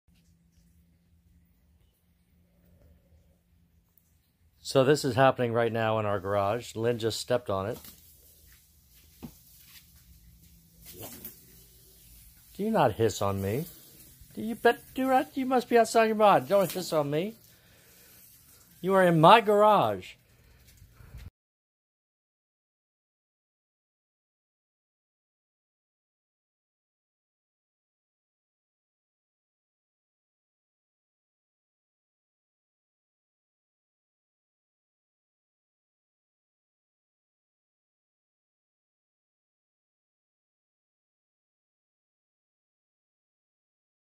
Clermont man uses broom to shoo gator out of his garage
A Clermont man said his wife stepped on a small alligator when she walked into the couple’s Clermont garage last weekend.